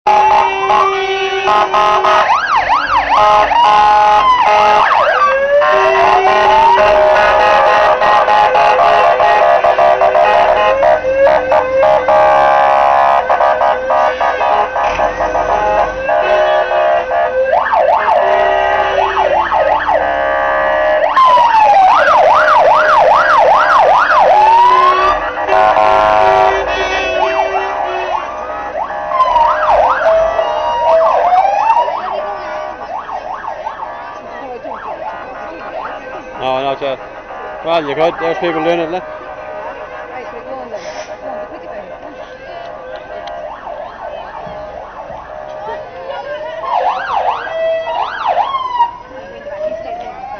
Fire Engines at St Patrick's Day Parade Carrickmacross 2009
Just a sample of the parade which was very good indeed!